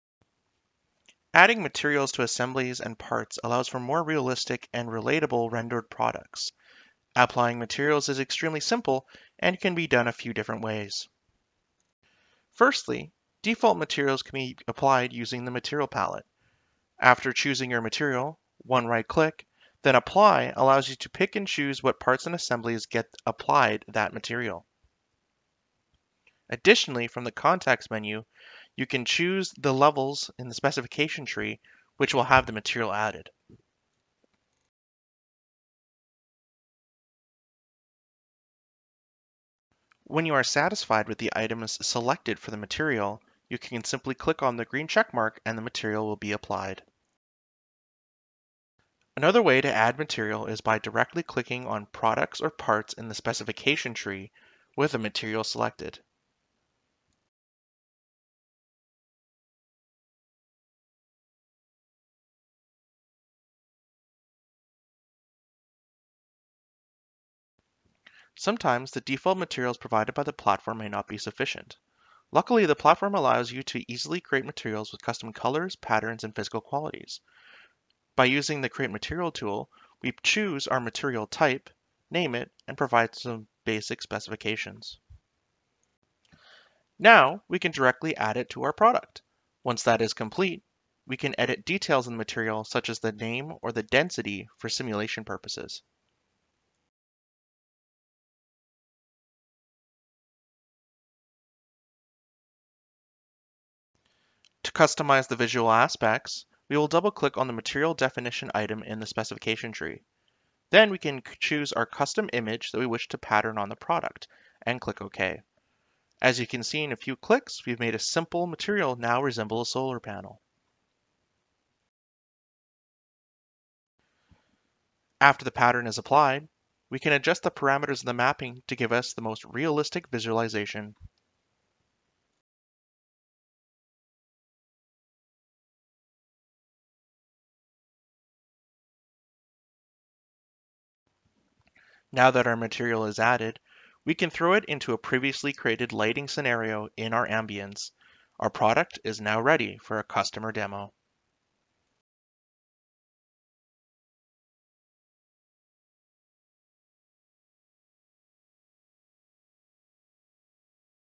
Preset and Custom Material definition with CATIA Live Rendering in 3DEXPERIENCE
In the age of virtual presentations dominating equipment and automation sales and marketing, create beautiful animations and realistic rendering of your 3D CAD models to present to customers. With CATIA Live rendering you can use preset materials and lighting conditions or create your own!